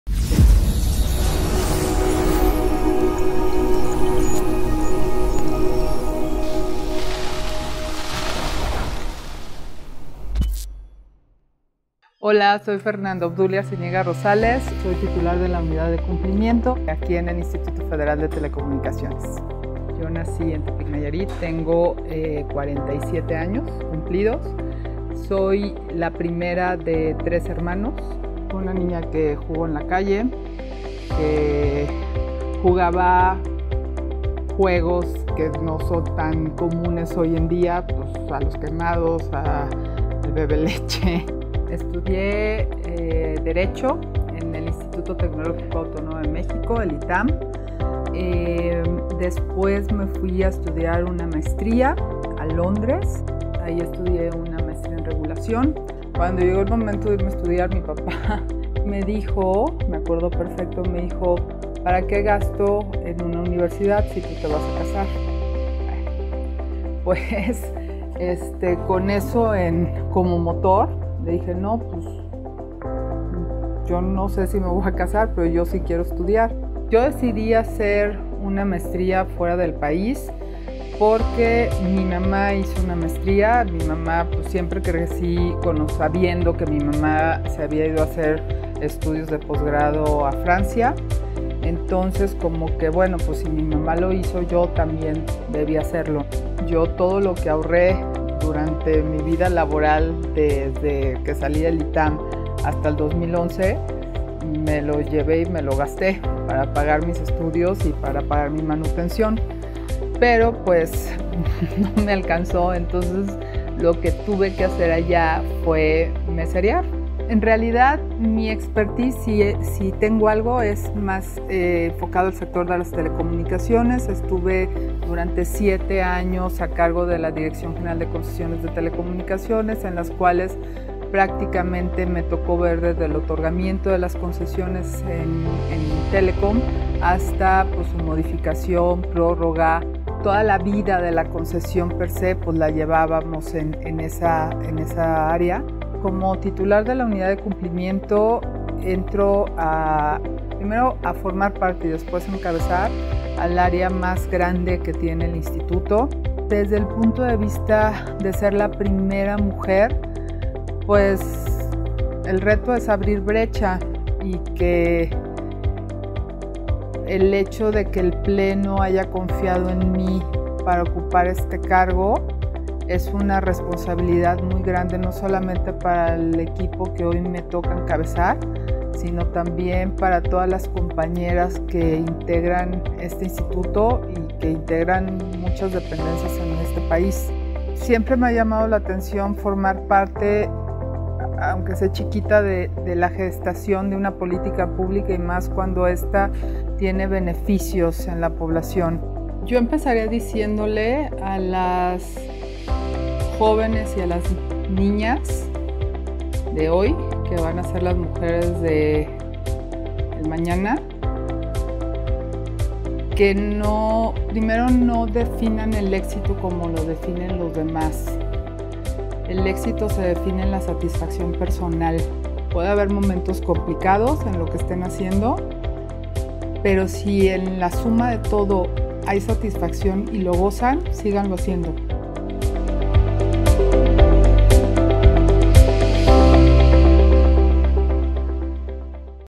Expertas que transforman 4 con Fernanda Arciniega
Te invitamos a conocer a @FernandaArcin15, primera mujer en ser la Titular de la Unidad de Complimiento del @IFT_MX 👩‍💻✅.
Ella es una de nuestras ExpertasQueTransforman 💜✨, conoce su historia.